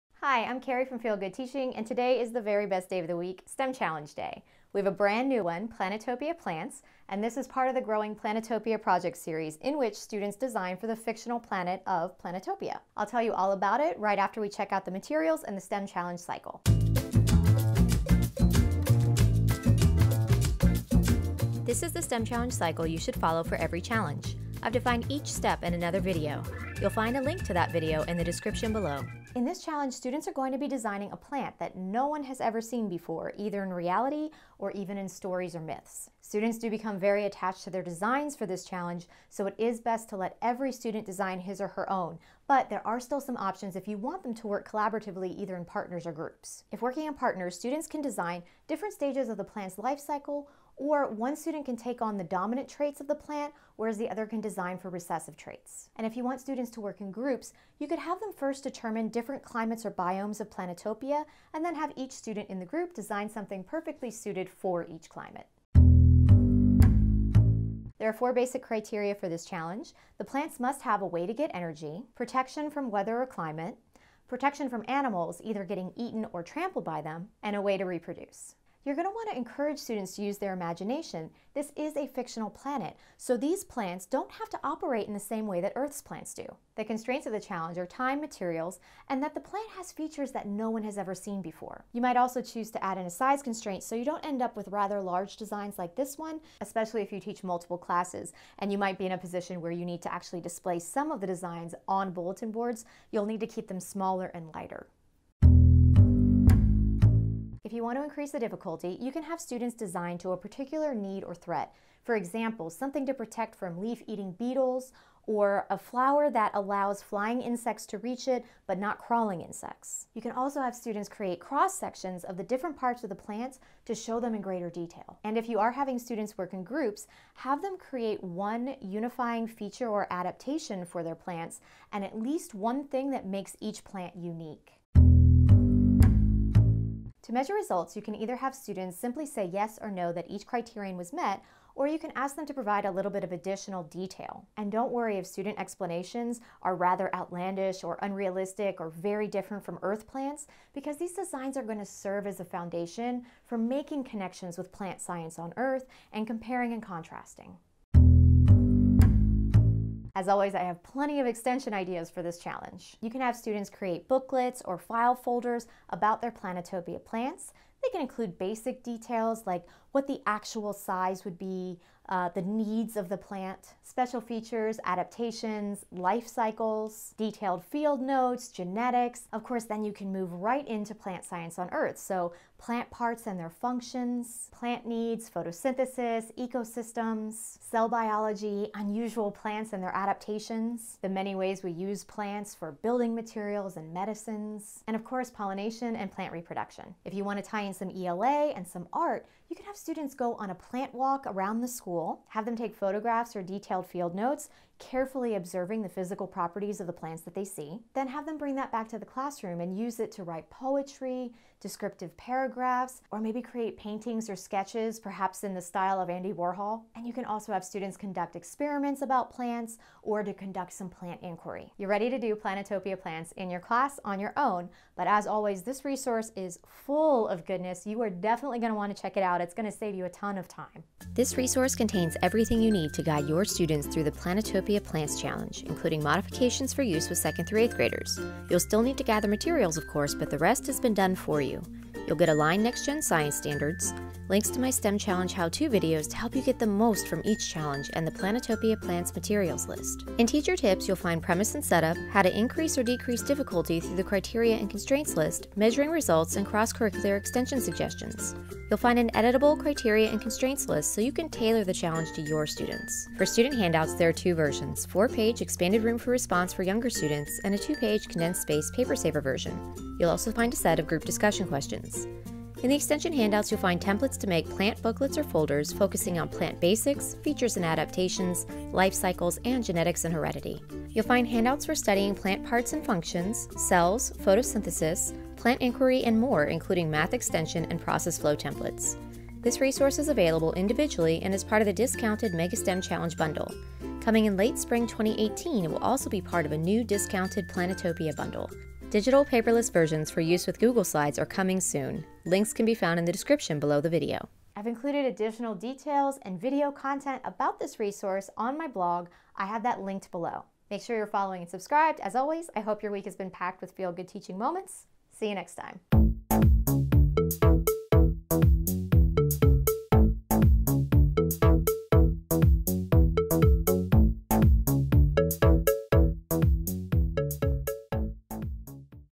Planetopia Plants STEM Challenge
If you’re looking to apply learning in plant needs, parts/functions, adaptations, life cycles, cells, photosynthesis, and/or genetics & heredity, this STEM / STEAM challenge is a perfectly engaging way to get the job done joyfully!

The challenge is great any time of the year, but works especially well around spring! The completed designs and extension projects make beautiful displays for Back to School Night or Open House as well!